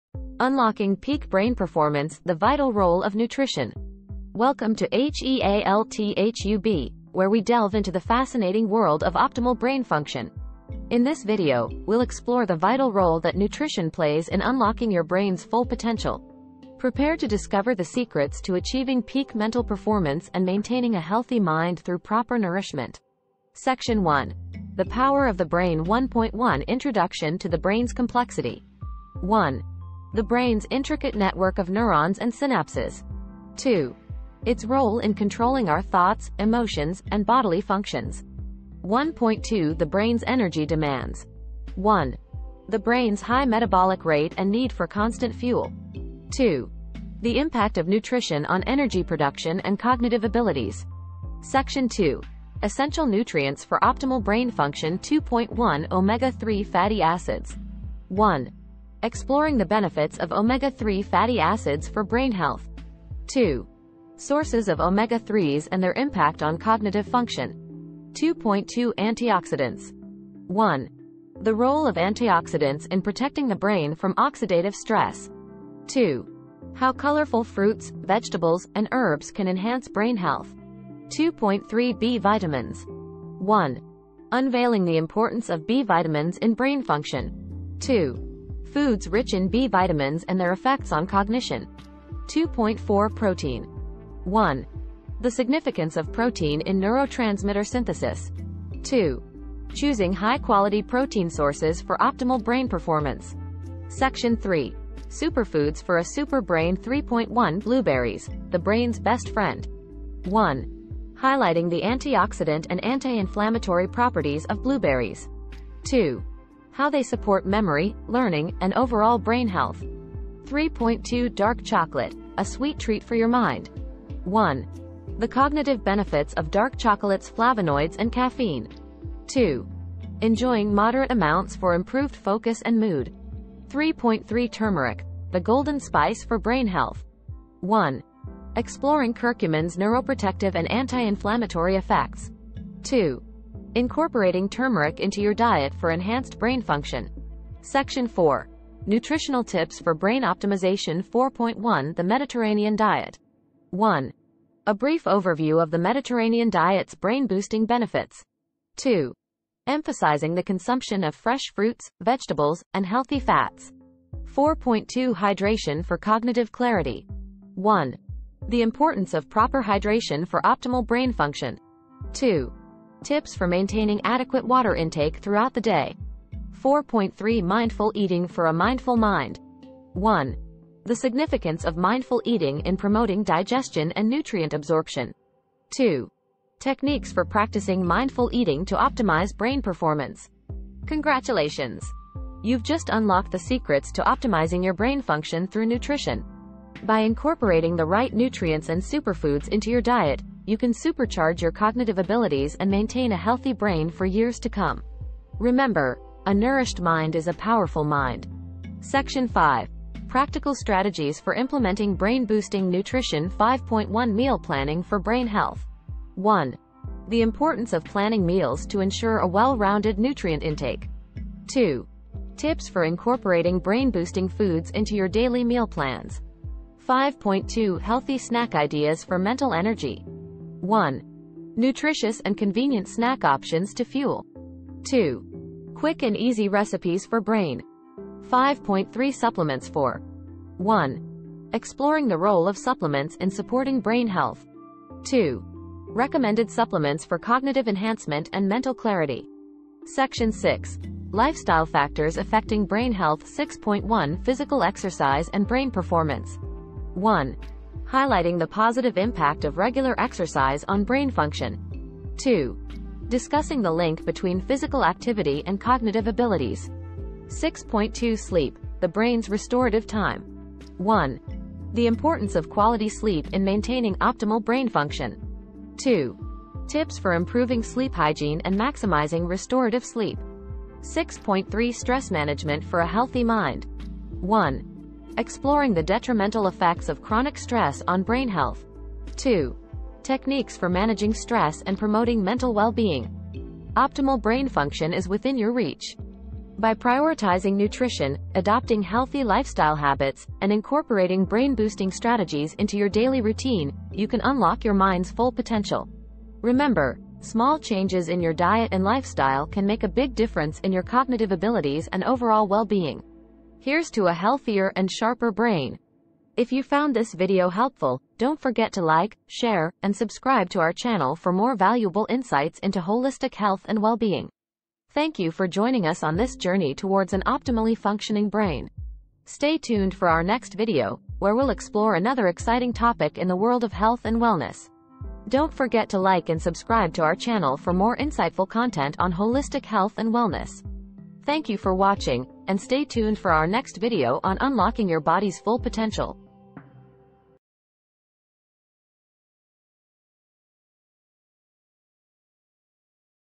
Nourish Your Brain, Enhance Your Life: The Vital Role of Nutrition in Optimal Brain Function
Discover the vital role that nutrition plays in enhancing brain function and overall quality of life. This comprehensive guide uncovers the impact of key nutrients, antioxidants, and dietary patterns on brain health. Learn how to nourish your brain through a balanced diet, supporting memory, focus, and emotional well-being. Elevate your cognitive performance and live your best life.